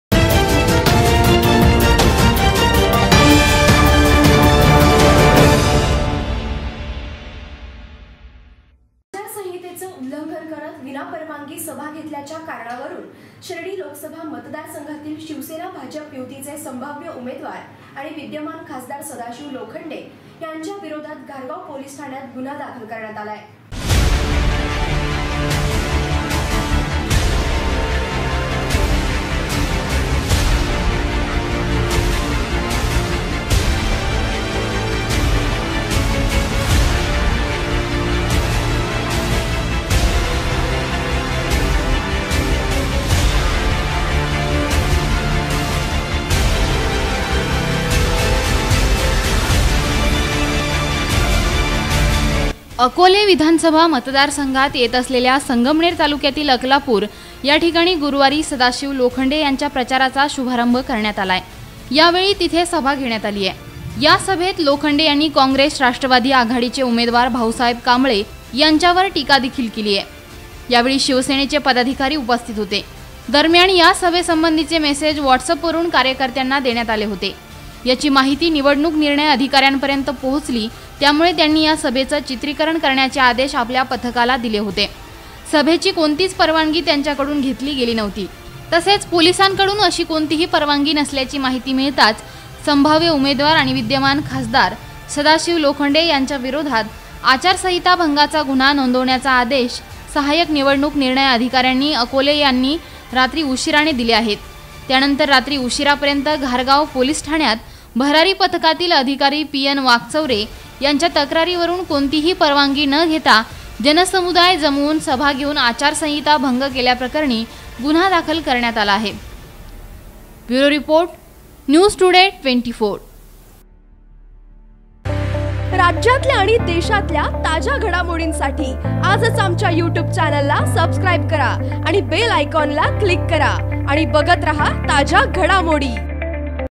आचारसंहिता भंग केल्याप्रकरणी खासदार सदाशिव लोखंडे यांच्यावर गुन्हा
आचारसंहितेचे उल्लंघन करत विनापरवानगी सभा घेतल्याच्या कारणावरुन शिर्डी लोकसभा मतदारसंघातील शिवसेना-भाजप युतीचे संभाव्य उमेदवार आणि विद्यमान खासदार सदाशिव लोखंडे यांच्या विरोधात घारगाव पोलिस ठाण्यात गुन्हा दाखल करण्यात आला आहे.